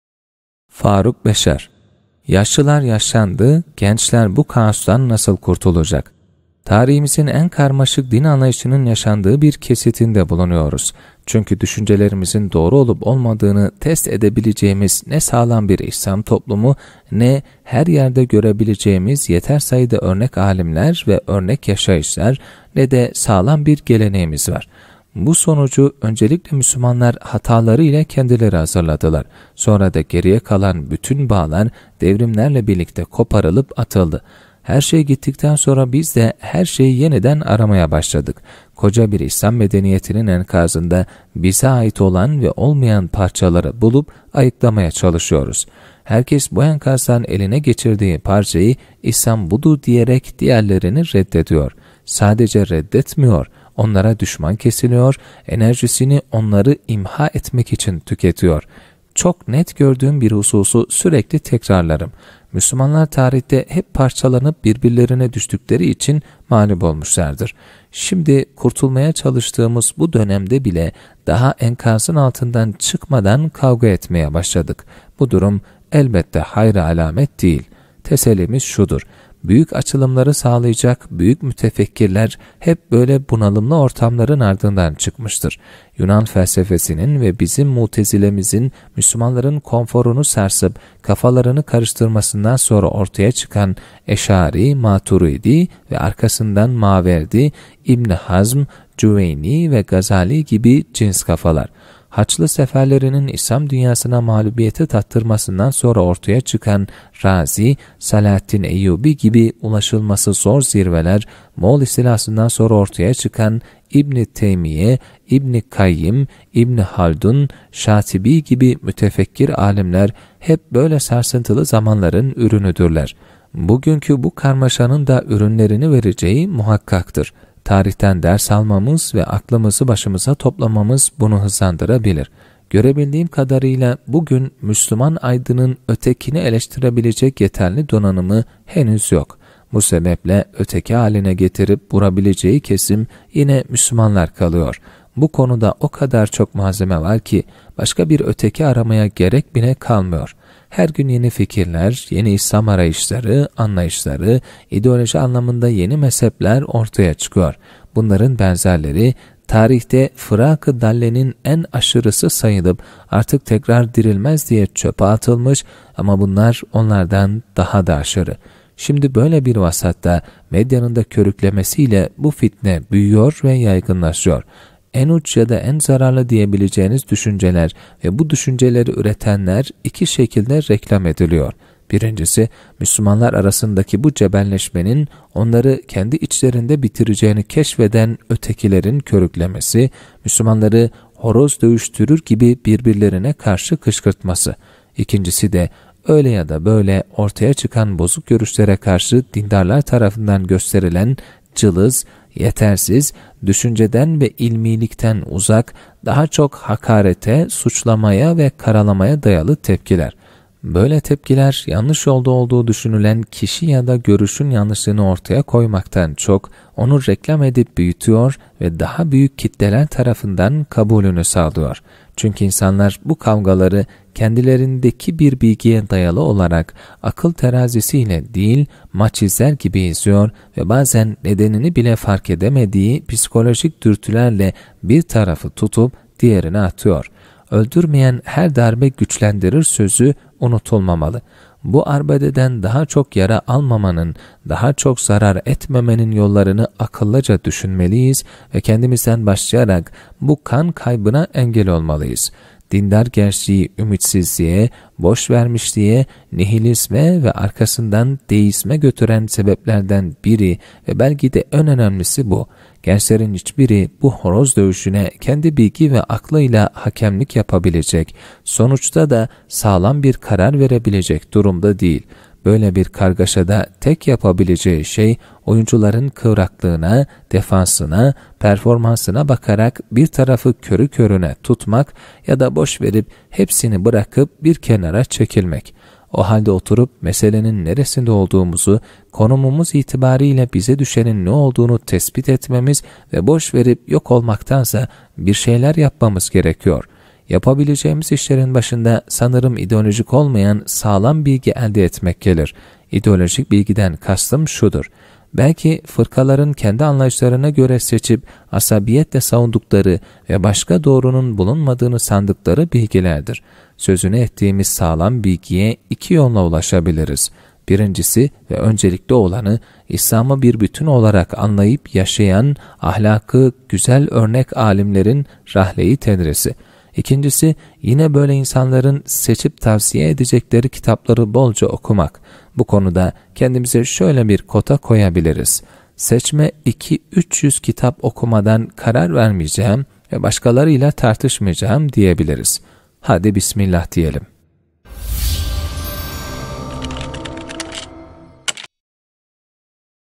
Faruk Beşer - Yaşlılar yaşlandı, gençler bu kaostan nasıl kurtulacak? - 01.02.2019
Tarihimizin en karmaşık din anlayışının yaşandığı bir kesitinde bulunuyoruz. Çünkü düşüncelerimizin doğru olup olmadığını test edebileceğimiz ne sağlam bir İslam toplumu, ne her yerde görebileceğimiz yeter sayıda örnek âlimler ve örnek yaşayışlar, ne de sağlam bir geleneğimiz var. Bu sonucu öncelikle Müslümanlar hatalarıyla kendileri hazırladılar. Sonra da geriye kalan bütün bağlar devrimlerle birlikte koparılıp atıldı. Her şey gittikten sonra biz de her şeyi yeniden aramaya başladık. Koca bir İslam medeniyetinin enkazında bize ait olan ve olmayan parçaları bulup ayıklamaya çalışıyoruz. Herkes bu enkazdan eline geçirdiği parçayı İslam budur diyerek diğerlerini reddediyor. Sadece reddetmiyor, onlara düşman kesiliyor, enerjisini onları imha etmek için tüketiyor.